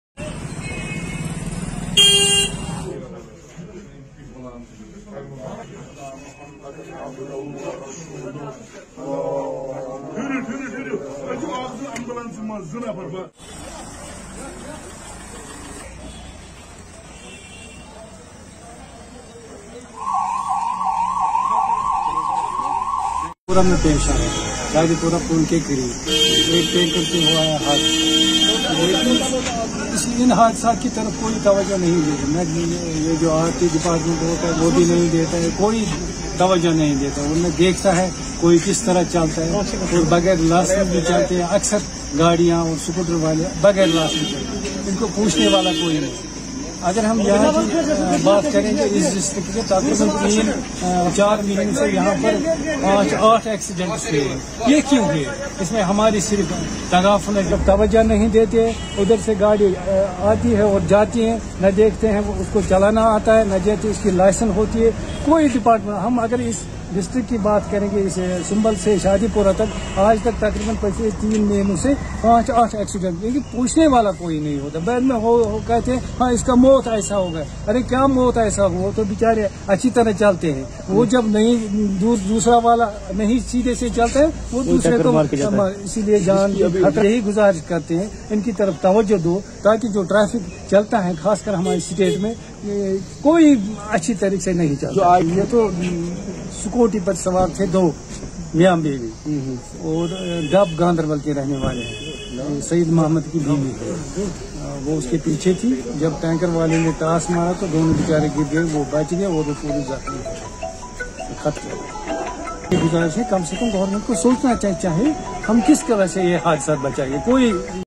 Woman killed, her husband injured in accident on Srinagar-Bandipora highway*Bandipora
Woman killed, her husband injured in accident on Srinagar-Bandipora highway*
Bandipora, Aug 28 : A woman was killed, while her husband was critically injured in a road accident near Shadipora on Srinagar-Bandipora highway on Sunday morning.